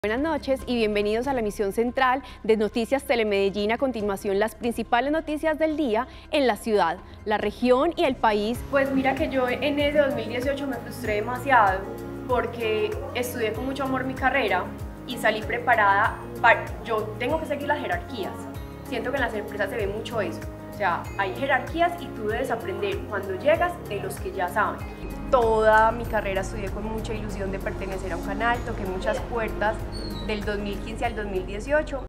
Melissa Escobar [Somos Emocionantes] Telemedellín
Melissa Escobar hace parte de los 20 preseleccionados de TelemedellínAcademy